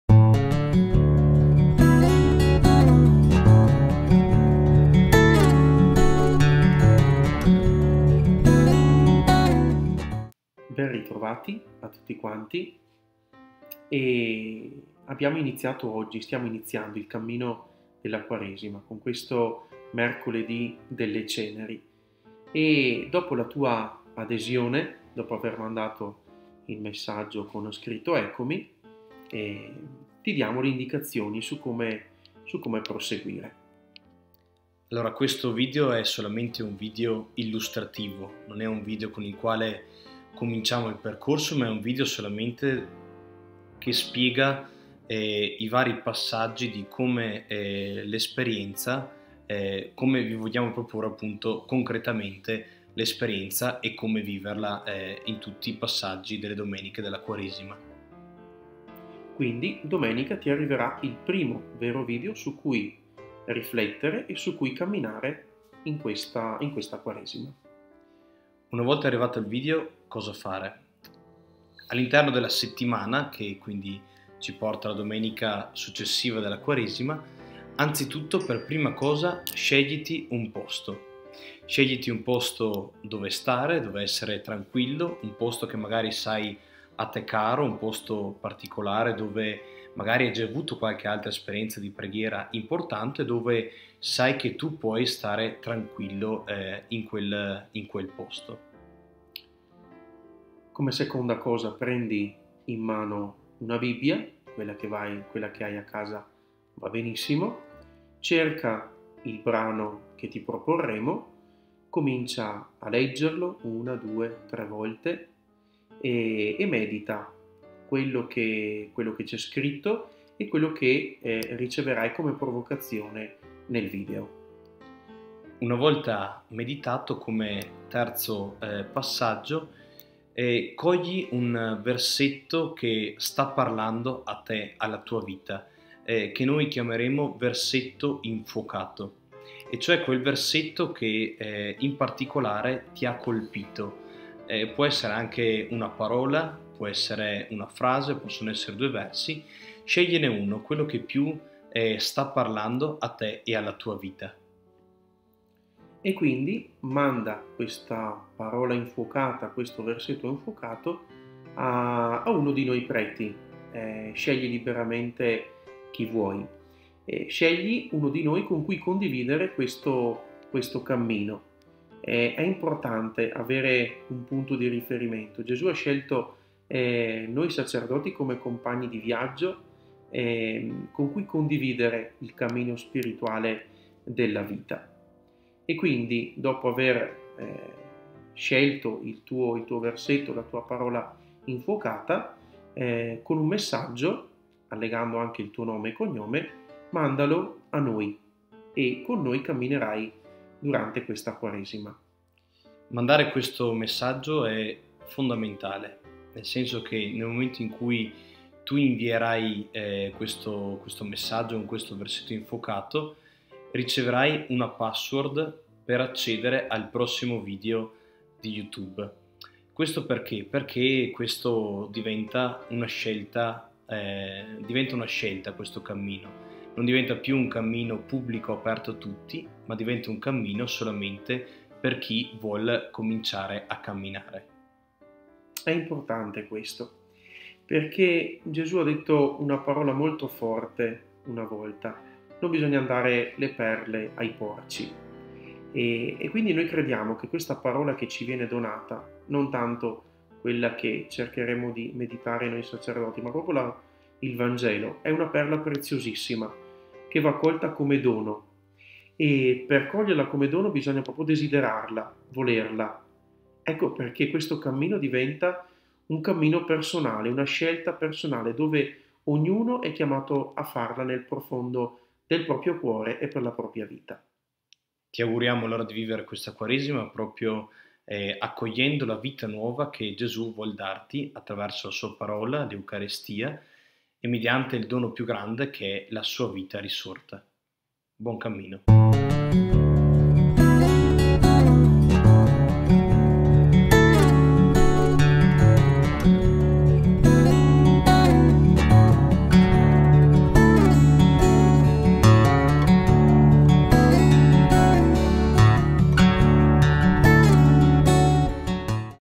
Quaresima 2017 : Come proseguire nel cammino?
Ecco il primo video in cui spieghiamo la struttura del percorso. Grazie per il vostro ECCOMI !!!